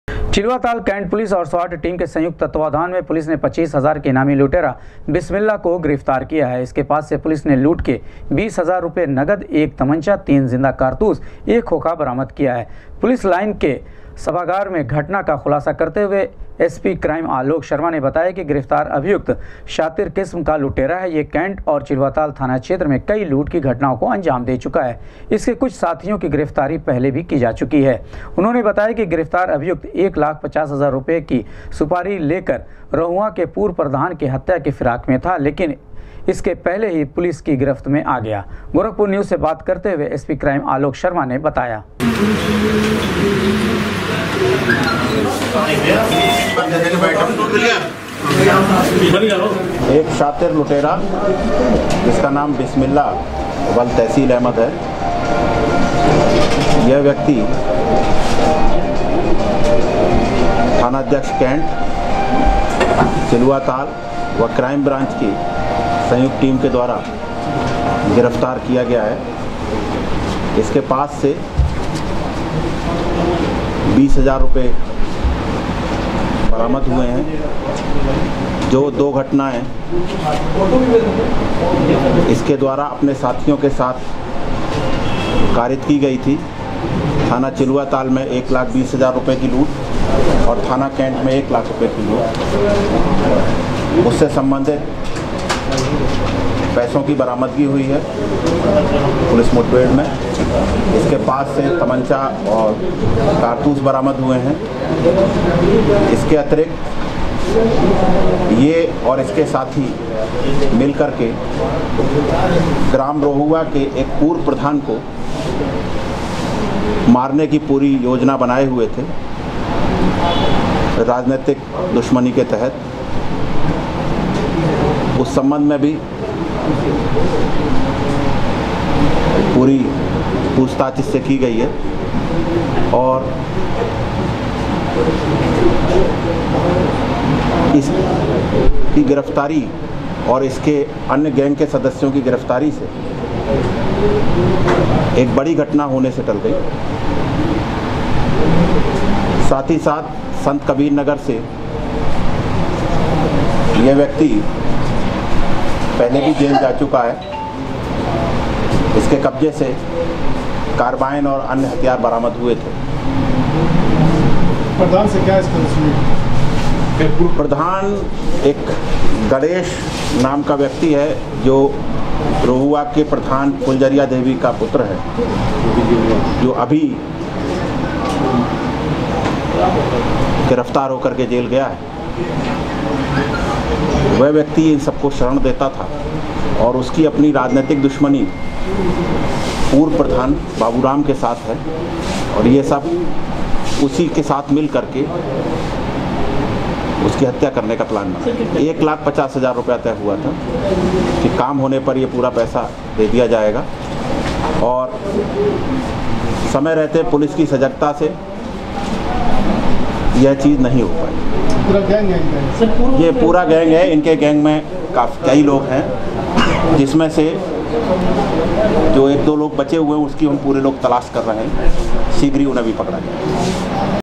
कैन्ट थाना क्षेत्र से 25 हजार का इनामी लुटेरा पुलिस मुठ़भेड़ में गिरफ्तार, एसपी क्राइम ने किया
कैन्ट थाना क्षेत्र से 25 हजार का इनामी लुटेरा पुलिस मुठ़भेड़ में गिरफ्तार, एसपी क्राइम ने किया खुलासा